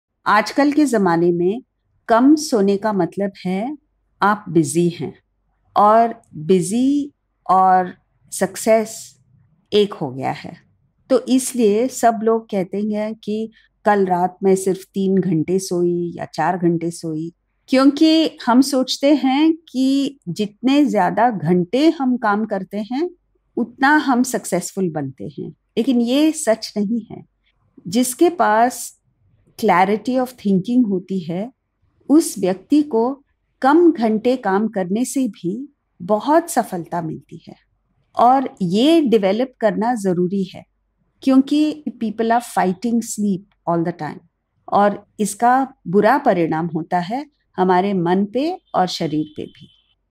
Is Sleep Important? by Jaya Row
Today, people wear sleep deprivation like a badge of honour.

They proudly talk about sleeping for just a few hours because they think being busy is the same as being successful.

In the latest episode of the GITA FOR THE YOUNG & RESTLESS podcast, JAYA ROW explains how sleep is integral to becoming a success!